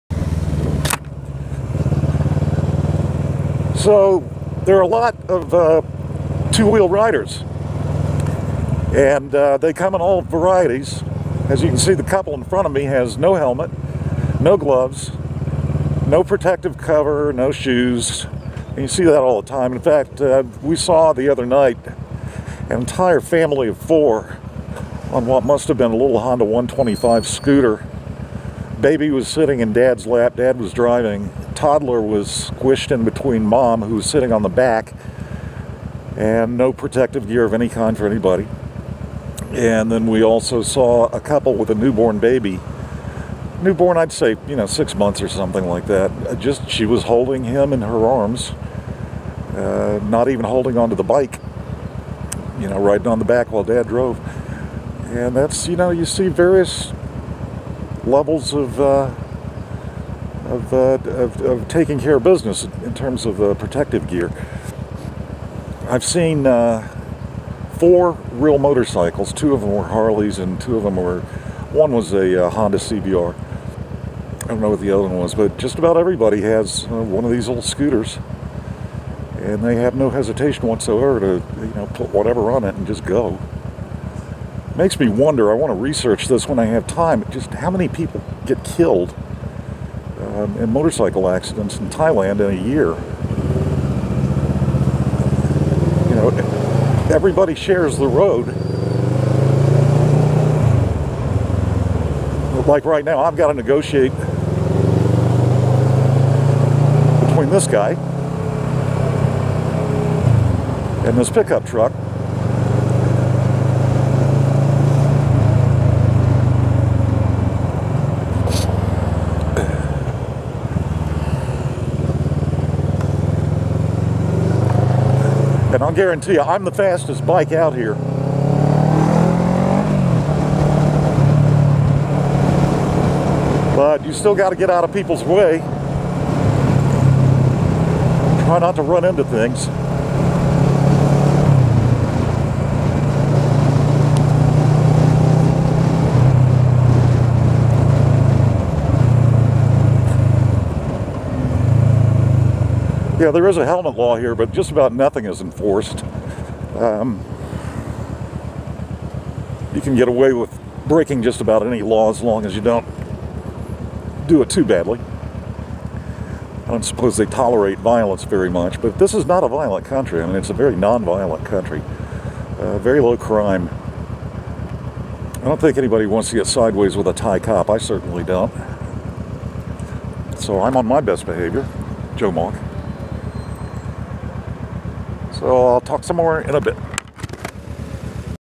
Chiang Mai 2016 riding out
Dealing with large hunks of metal and small pieces of bikes